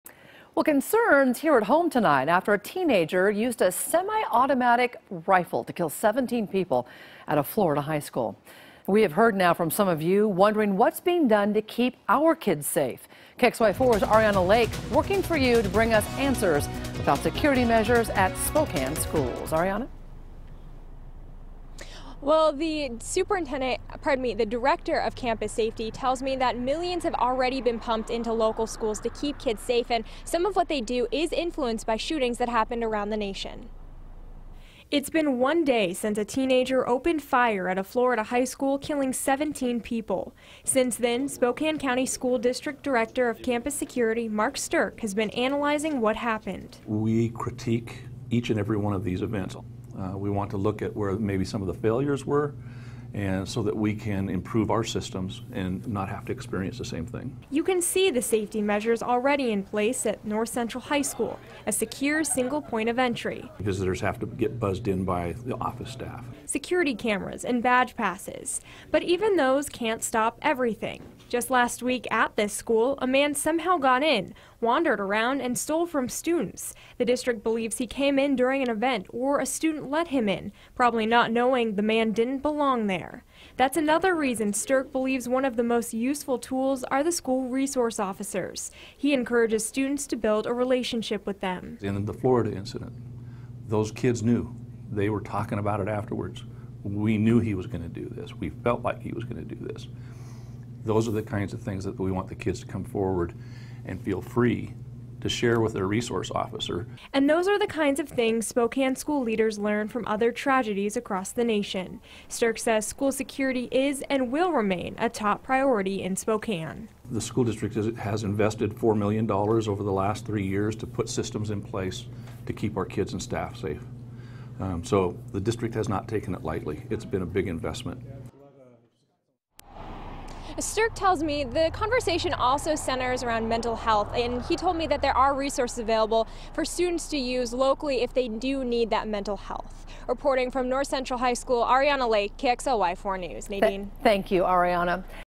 Spokane County School District learns from Florida shooting
The school shooting in Florida has sparked more concern and questions from local parents about what's being done locally to protect students.  KXLY4's Ariana Lake reports.